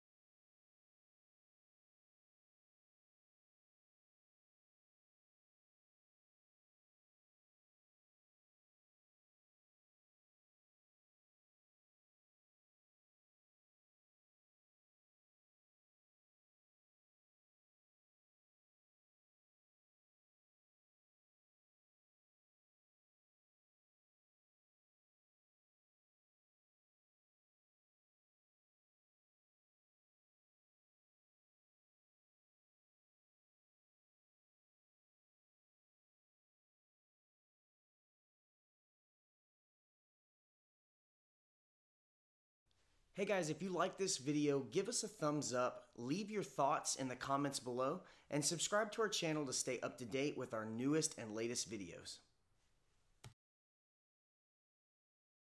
FIX YOUR Roundoff Back Handspring FULL
This variation of the bridge ties together the glute,  hamstrings, and rotation muscles of the core.  It builds coordination and strength and can help improve things like twisting activities as well as lower back pain!